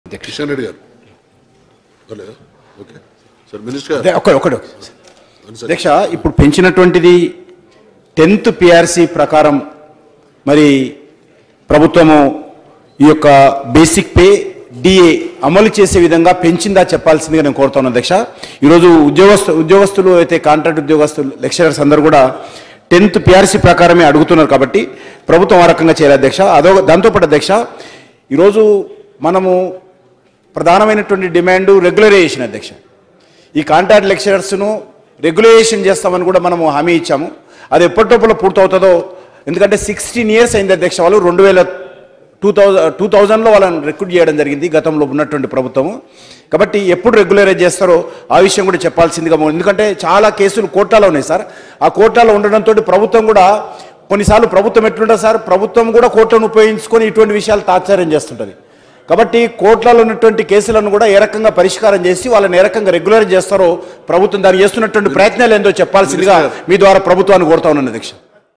MLA Kishan Reddy Raise Question On Regularization Of Contract Lecturers | TS Assembly | V6News
Telangana BJP MLA Kishan Reddy speaks on regularization and hike of contract lecturer salaries.